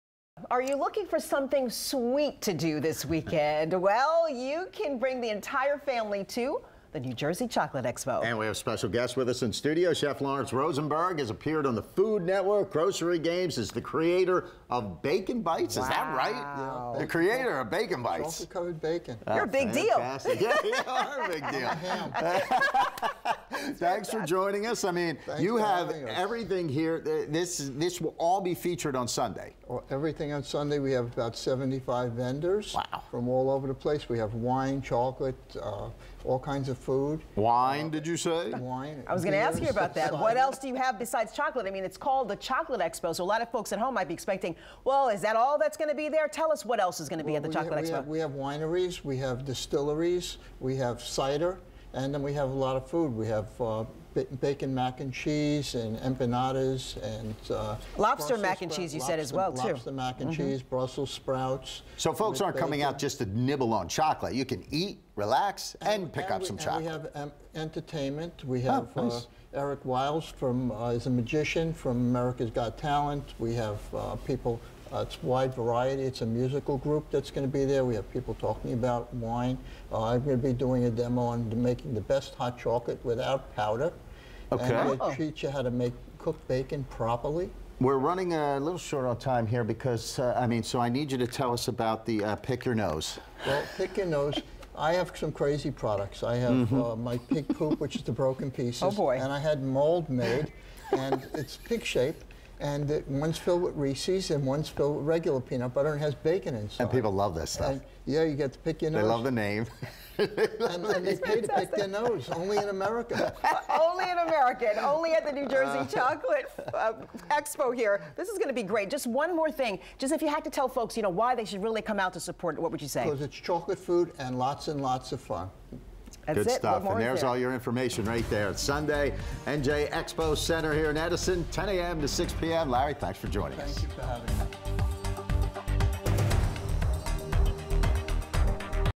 Food Network chef discusses NJ Chocolate Expo
Chef Lawrence Rosenberg, who you may know from Food Network’s “Guy’s Grocery Games,” stopped by News 12 New Jersey to give the inside scoop.